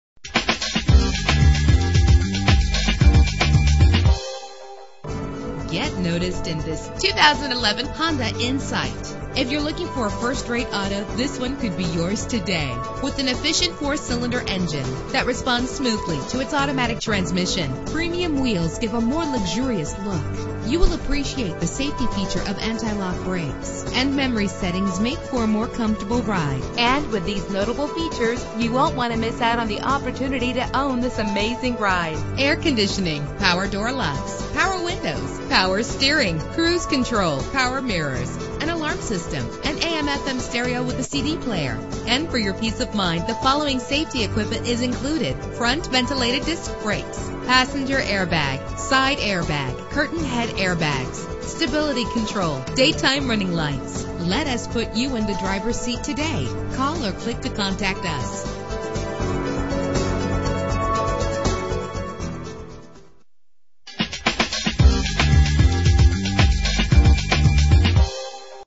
2011 Honda Insight - Nashua NH
Year: 2011
Make: Honda
Model: Insight
Trim: EX
Engine: 1.3 liter inline 4 cylinder 8 valve
Transmission: Automatic CVT
Color: Crimson Pearl
Mileage: 10
Address:
280-300 Amherst St